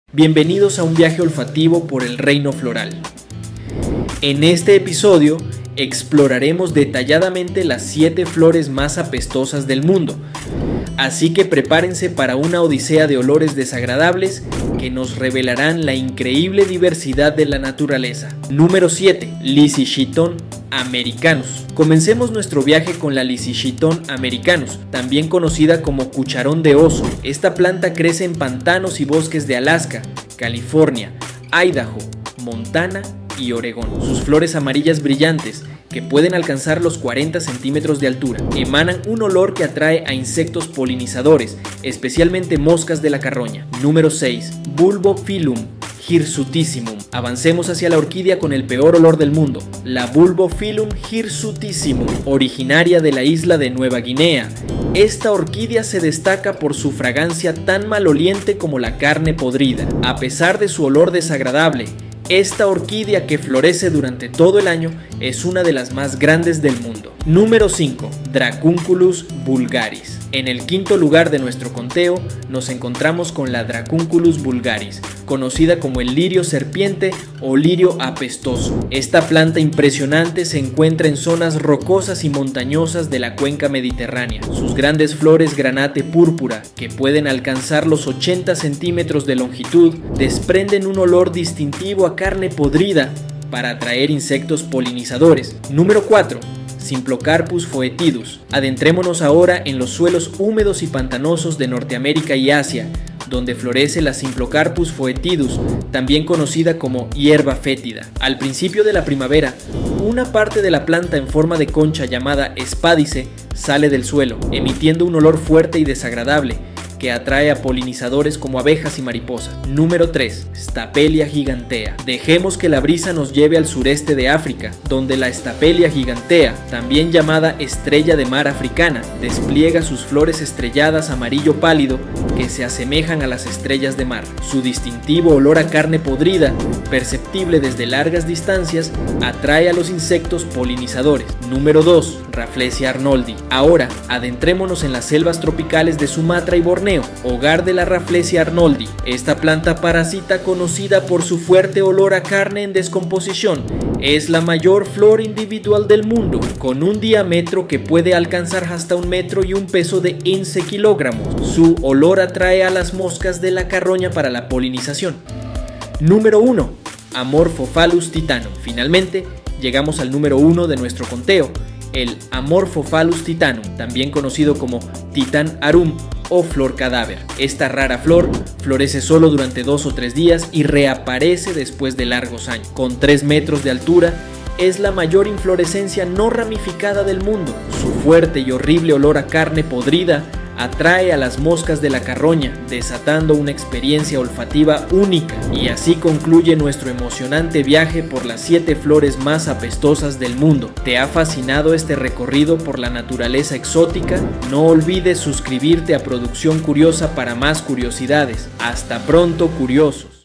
Las 7 Flores mas APESTOSAS del MUNDO 🌹
La mayoría de las flores tienen olores agradables, aunque hay un grupo de plantas que huelen mal. ¿El motivo? Al parecer, el olor nauseabundo es necesario para atraer a los insectos polinizadores con mayor facilidad.